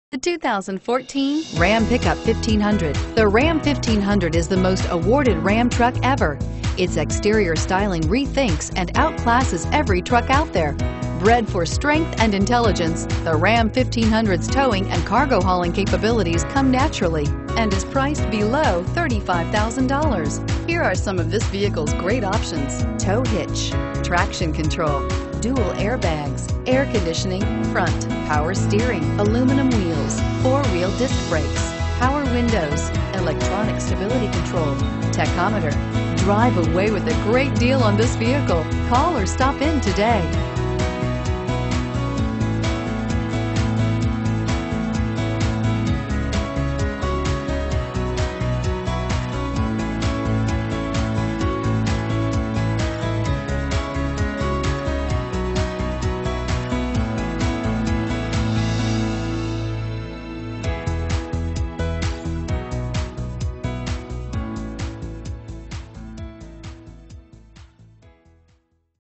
2014 Dodge Ram 1500 Ankeny Des Moines Ames Iowa D141593
2014 Dodge Ram 1500 Tradesman

3.55 Rear Axle Ratio and 20X9.0 Aluminum Chrome Clad Wheels. Popular Equipment Group (SIRIUSXM Satellite Radio), 4WD, and Anti-Spin Differential Rear Axle. You'll be hard pressed to find a better truck than this dependable 2014 Dodge Ram 1500. Sometimes the fun doesn't begin until the pavement ends, which helps make the off-road ability of this Ram 1500 Tradesman so appealing.